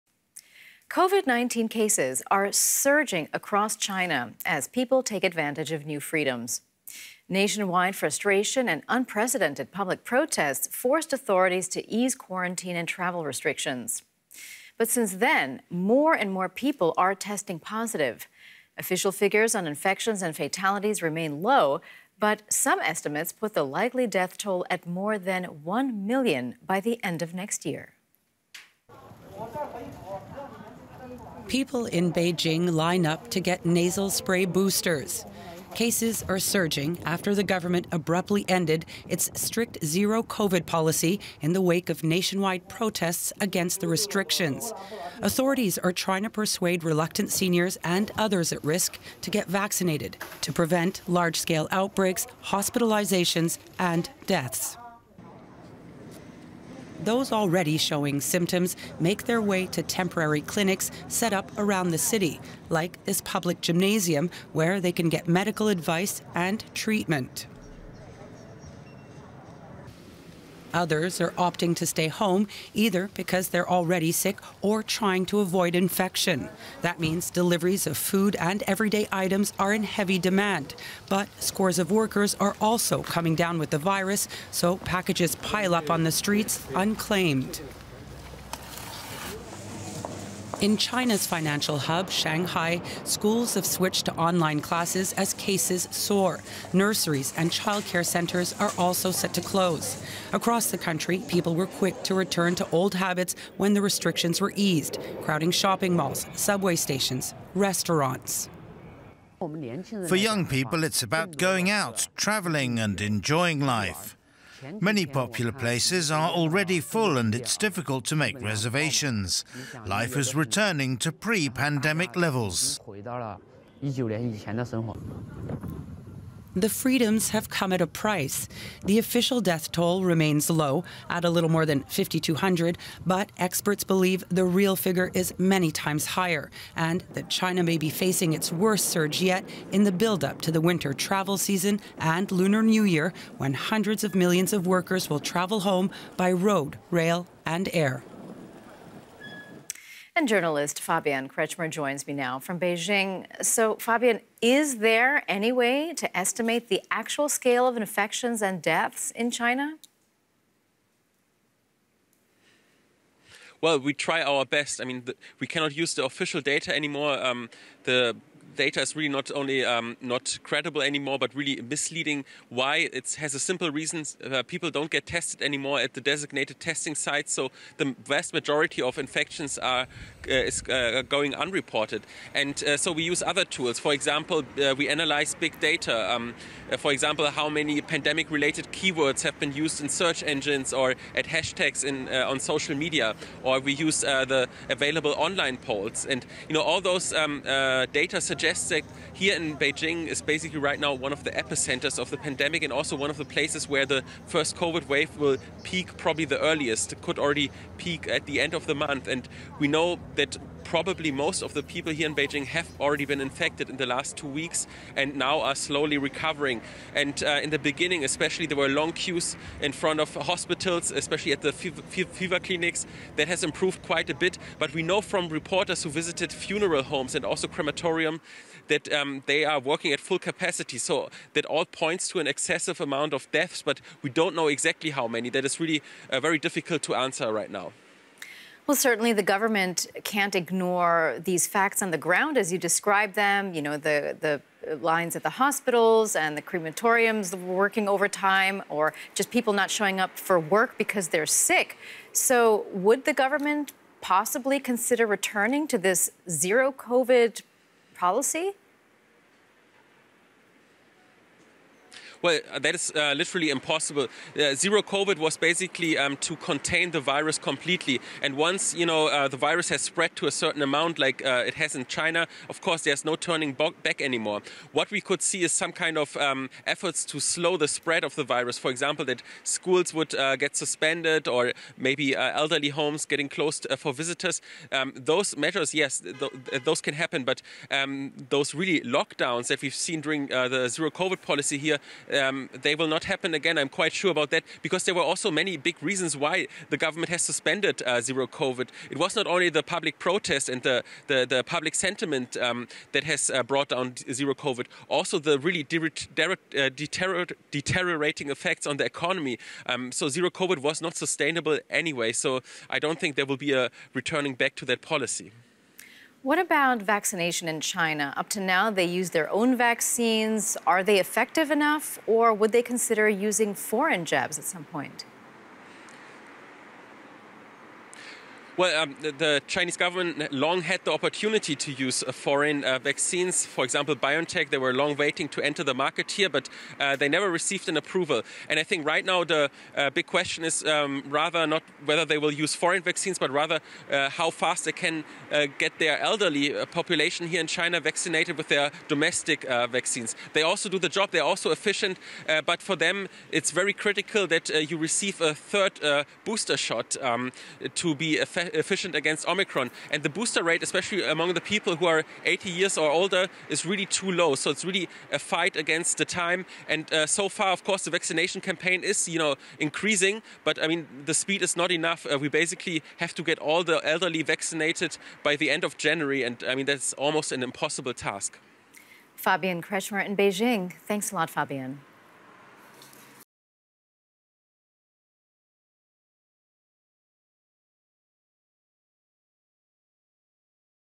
What can China do to slow its post-lockdown COVID surge? | DW News
COVID-19 cases are surging across China as people take advantage of new freedoms. Nationwide frustration and unprecedented public protests forced authorities to ease quarantine and travel restrictions. But since then more and more people are testing positive. Official figures on infections and fatalities remain low, but some estimates put the likely death toll at more than a million by the end of next year.